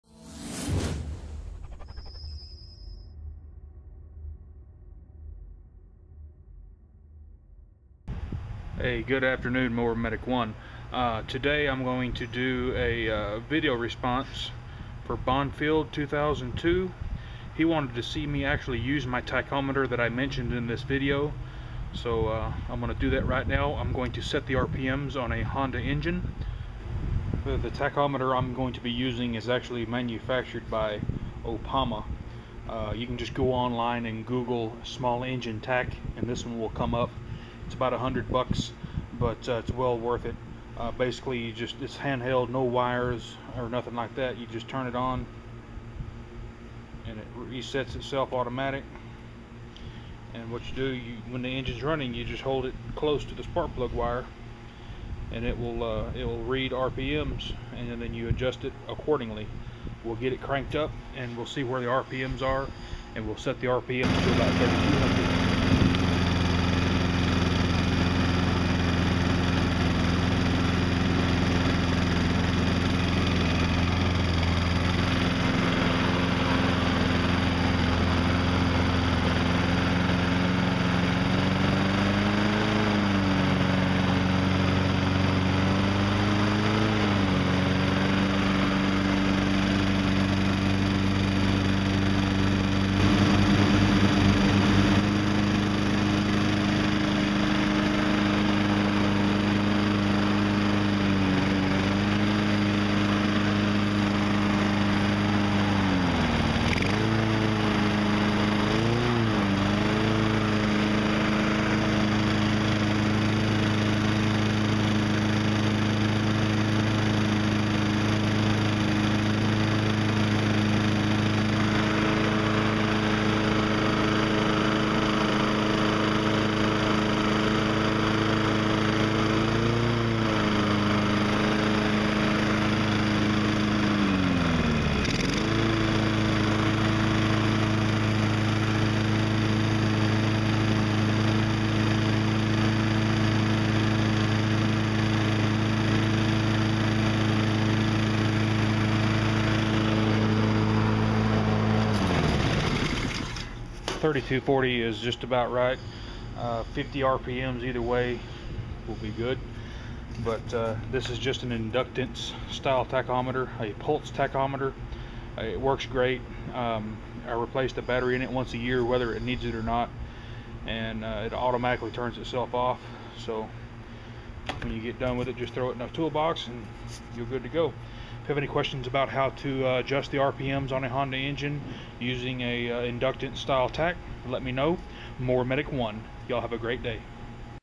VIDEO REQUEST FOR BONFIELD2002 how to set engine rpm's using an inductance style tachometer
JEREMY FOUNTAIN
THEMOWERMEDIC1

TREY DRIVER
BIG BUCK MOWERS AND EQUIPMENT
3301 STATE HIGHWAY 121
BONHAM TEXAS 75418

watch as the mowermedic1 uses a tach to set the rpm on a small engine